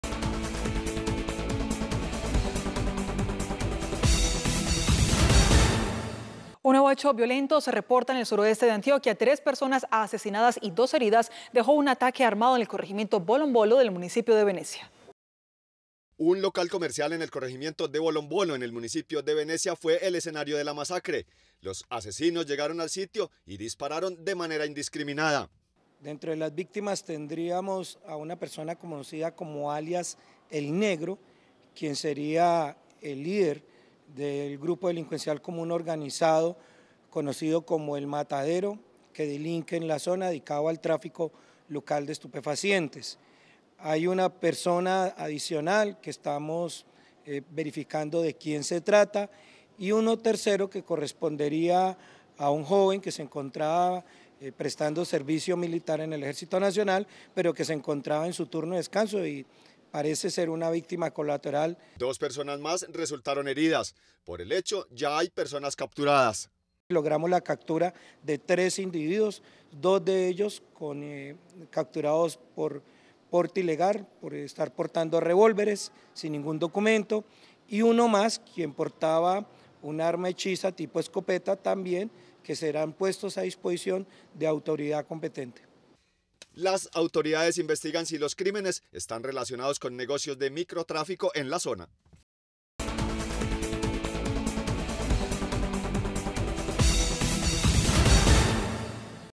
Tres personas muertas y una herida deja ataque armado en Venecia - Teleantioquia Noticias
Un nuevo hecho violento se reporta en el Suroeste de Antioquia. Tres personas asesinadas y dos heridas, dejó un ataque armado en el corregimiento Bolombolo del municipio de Venecia.